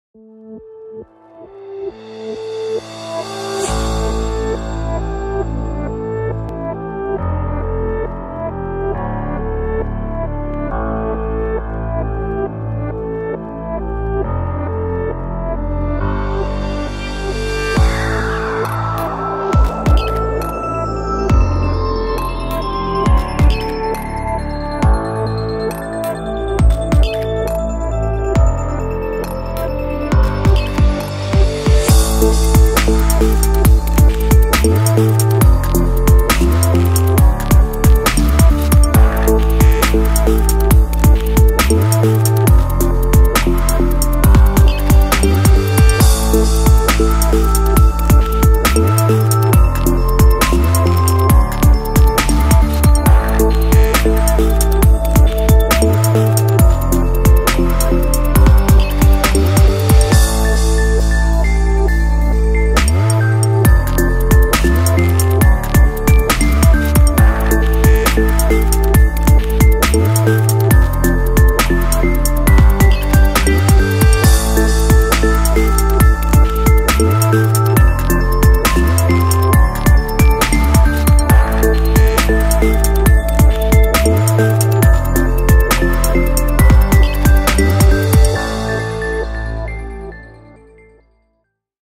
HAVAŞ SUMMIT 2019
Havaş Liderleri ile Havaş Summit2019'da buluştuk. / We met with Havaş Leaders at Havaş Summit 2019